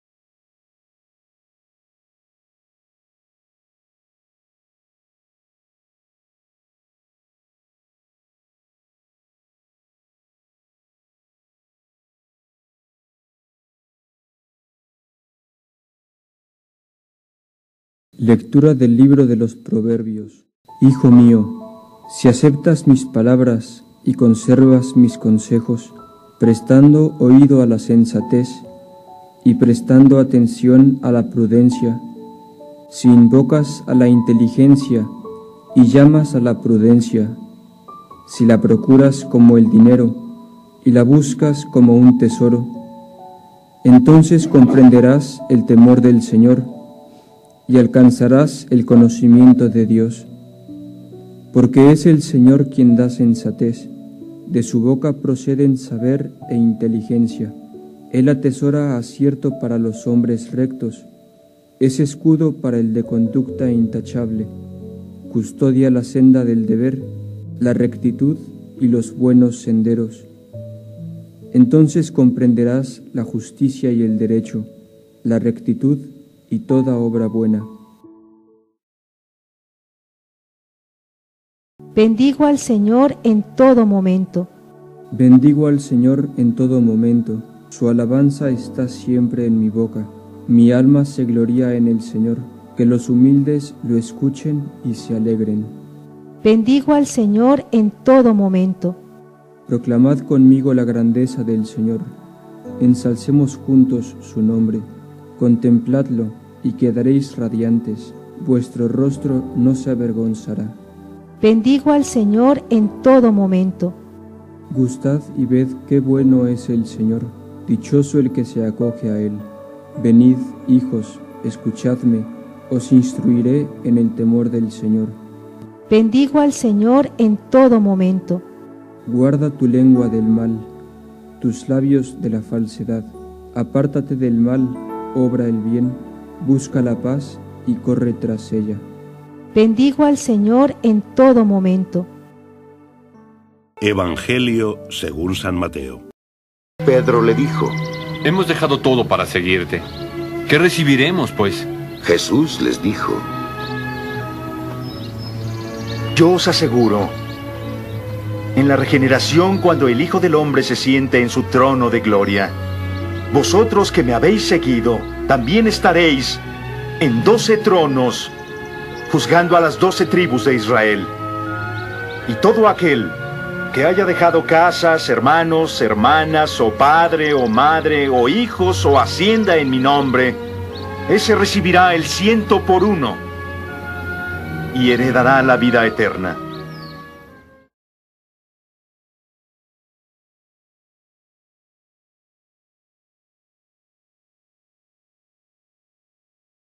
Fiesta San Benito Abad Julio 11 de 2020 Patrono de Europa Mateo 19,27-29
Audio Video Lecturas propias de la Fiesta de San Benito Abad Patrono de Europa 11 de Julio de 2020 Primera Lectura: Del libro los Proverbios 2,1-9 Salmo 33,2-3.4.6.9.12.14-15 R/. Bendigo al Señor en todo momento Del Santo Evangelio según San Mateo 19,27-29 SAN BENITO, ABAD, PATRONO DE EUROPA, Fiesta 2020:
PRIMERA LECTURA DE LA MISA
Presta atención a la prudencia
Lectura del libro de los Proverbios 2, 1-9
Hijo mío, si aceptas mis palabras y conservas mis consejos, prestando oído a la sensatez y prestando atención a la prudencia; si invocas a la inteligencia y llamas a la prudencia; si la procuras como el dinero y la buscas como un tesoro, entonces comprenderás el temor del Señor y alcanzarás el conocimiento de Dios. Porque es el Señor quien da sensatez, de su boca proceden saber e inteligencia. Él atesora acierto para los hombres rectos, es escudo para el de conducta intachable, custodia la senda del deber, la rectitud y los buenos senderos. Entonces comprenderás la justicia y el derecho, la rectitud y toda obra buena.
SALMO RESPONSORIAL
Salmo responsorial
Salmo 33,2-3.4 y 6.9 y 12.14-15(R.:2a)
R/. Bendigo al Señor en todo momento.
Bendigo al Señor en todo momento, su alabanza está siempre en mi boca;
mi alma se gloría en el Señor: que los humildes lo escuchen y se alegren. R.
Proclamad conmigo la grandeza del Señor, ensalcemos juntos su nombre.
Contempladlo, y quedareis radiantes, vuestro rostro no se avergonzara. R.
Gustad y ved que bueno es el Señor, dichoso el que se acoge a él. Venid, hijos,
escuchadme: os instruiré en el temor del Señor. R.
Guarda tu lengua del mal, tus labios de la falsedad;apártate del mal, obra el bien,
busca la paz y corre tras ella. R.
EVANGELIO DE LA MISA
Vosotros, los que me habéis seguido, recibiréis cien voces más
En aquel tiempo, dijo Pedro a Jesús: -«Nosotros lo hemos dejado todo y te hemos seguido;¿qué nos va a tocar?» Jesús les dijo: -«Os aseguro: cuando llegue la renovación, y el Hijo del hombre se siente en el trono de su gloria, también vosotros, los que me habéis seguido, os sentaréis en doce tronos para regir a las doce tribus de Israel. El que por mí deja casa, hermanos o hermanas, padre o madre, mujer, hijos o tierras, recibirá cien veces más, y heredará la vida eterna. »